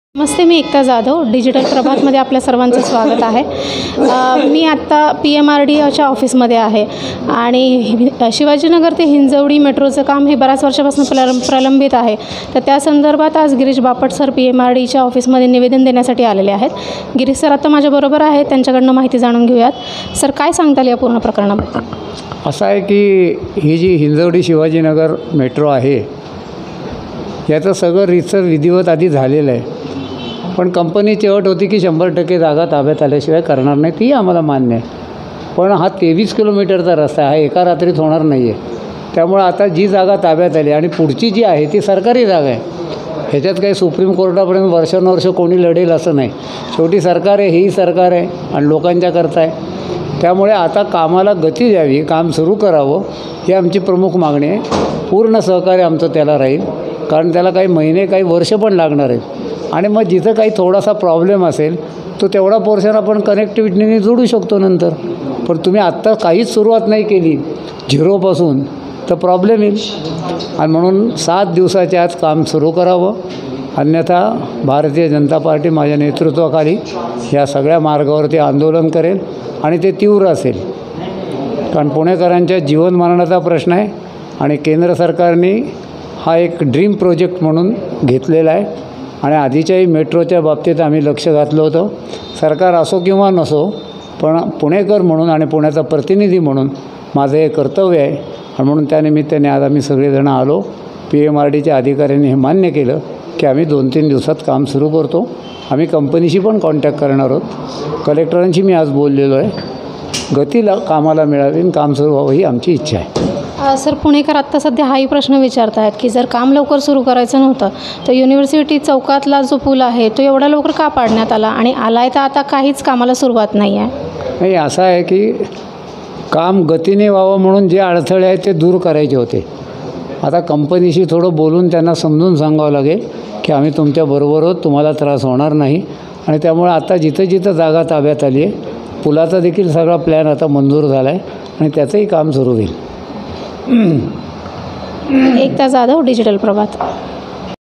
सात दिवसांच्या आत हिंजवडी ते शिवाजीनगर मेट्रोचे काम सुरू झाले नाही तर तीव्र आंदोलन करू - गिरीश बापट
सात दिवसांच्या आत हिंजवडी ते शिवाजीनगर मेट्रोचे काम सुरू झाले नाही तर तीव्र आंदोलन करू - गिरीश बापट(खासदार,भाजप)